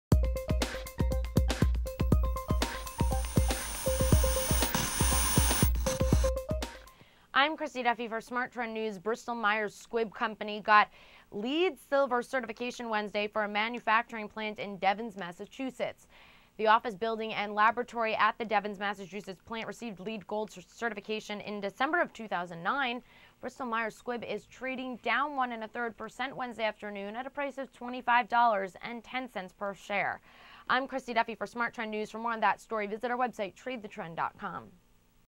News Update: Bristol-Myers Squibb Co. Received LEED Silver Certification for Devens, MA Plant
Bristol-Myers Squibb Co. (NYSE:BMY) got LEED Silver certification Wednesday for a manufacturing plant in Devens, Massachusetts.

The office building and laboratory at the Devens, MA plant received LEED Gold certification in December 2009.

Bristol-Myers Squibb is trading down 1.30% at $25.10.